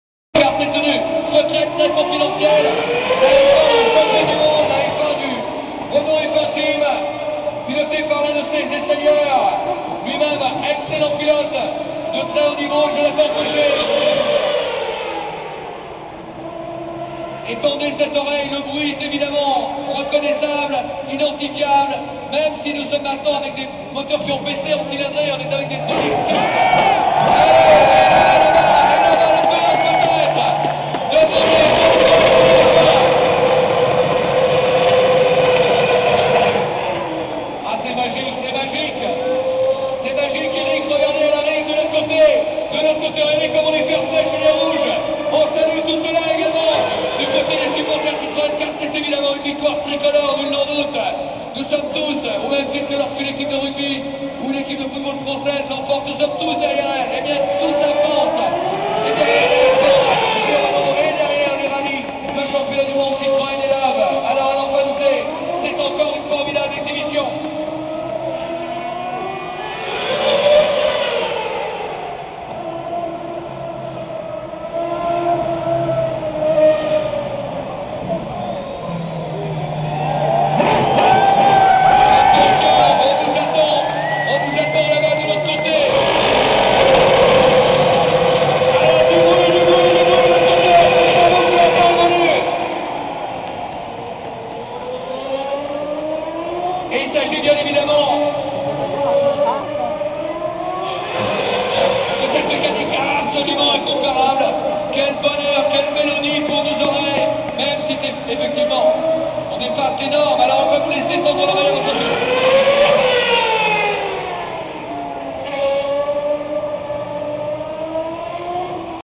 ROC 2006 : F1 R26
Exhibition de la Renault championne du monde de formule 1 2006, la R26 avec Jonathan Cochet au volant